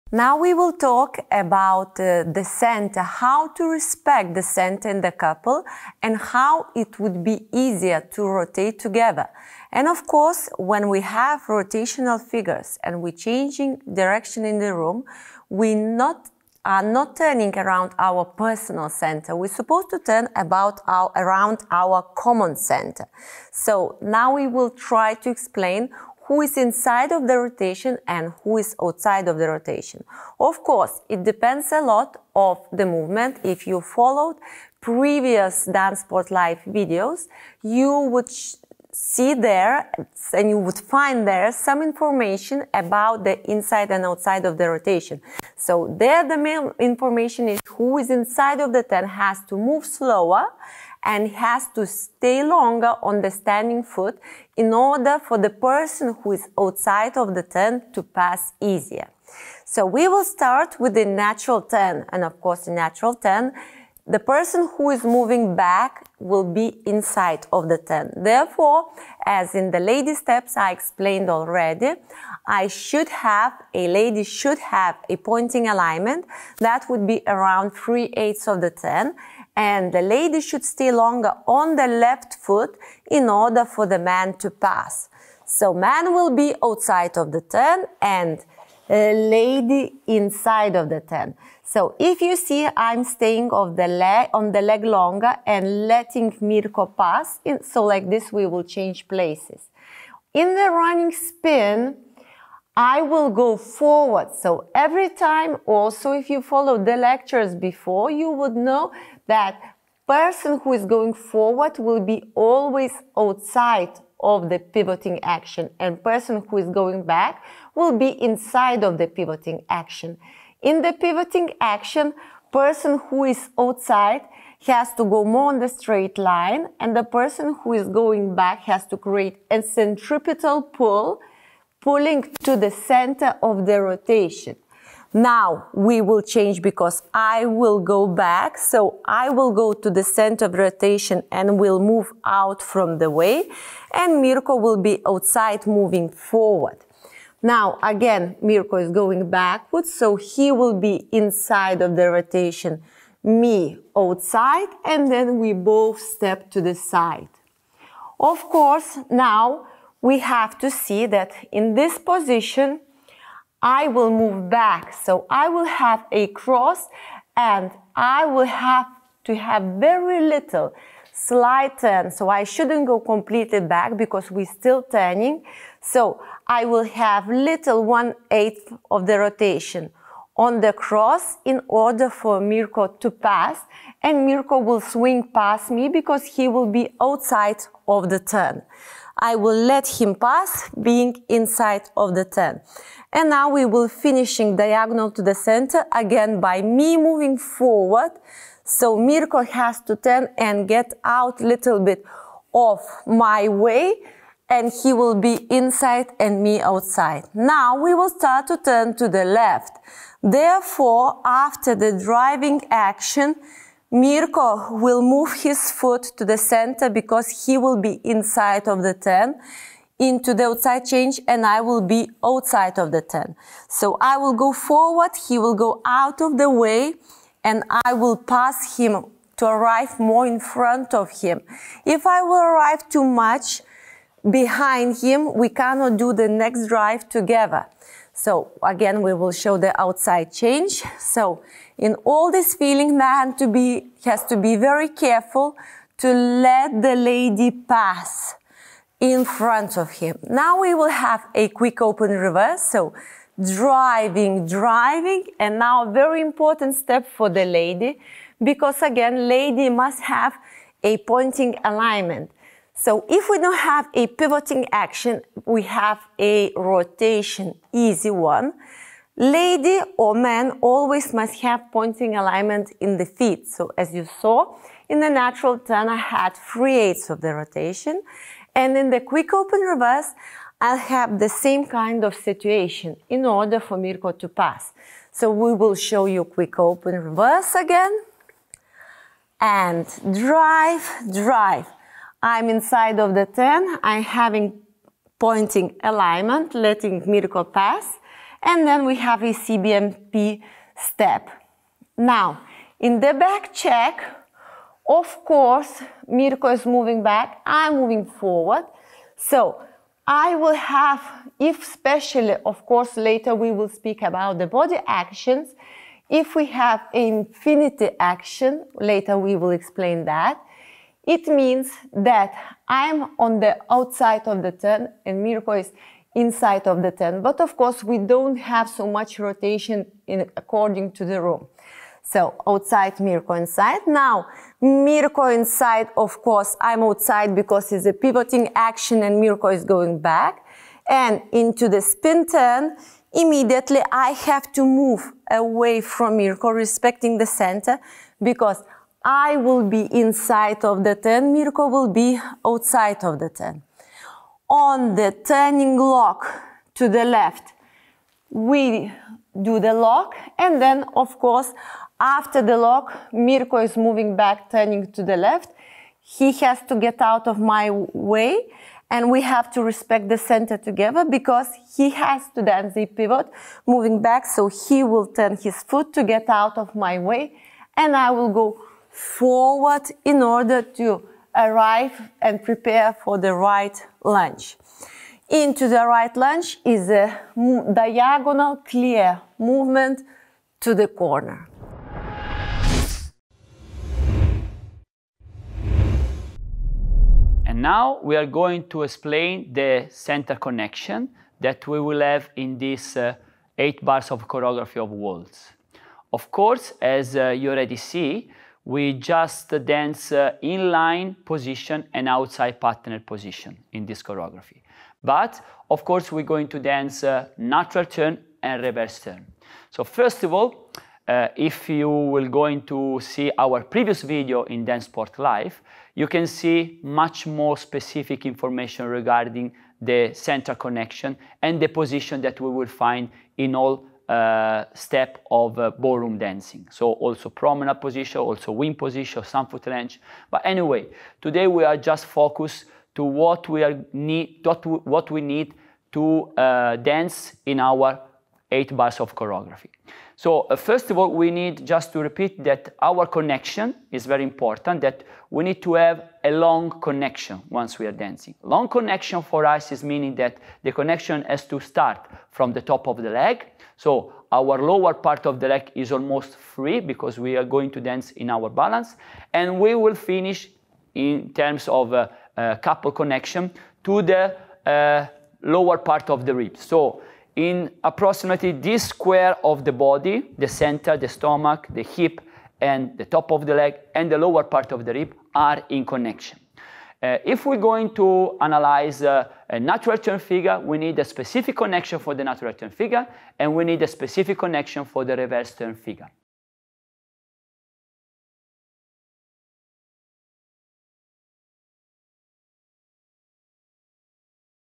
Mirko & Edita Gozzoli Explain The Technique Behind the Steps in Waltz Choreography
Short snippet from the new online course "Fashionable Choreographies & The Technique Behind Them" by Mirko & Edita Gozzoli.

In their new online course, Mirko & Edita Gozzoli are teaching you some easy & fashionable choreographies & the techniques behind them.

You will learn everything you need to know about these routines: men & ladies figure name, timing, footwork, body & head actions, and of course, connection.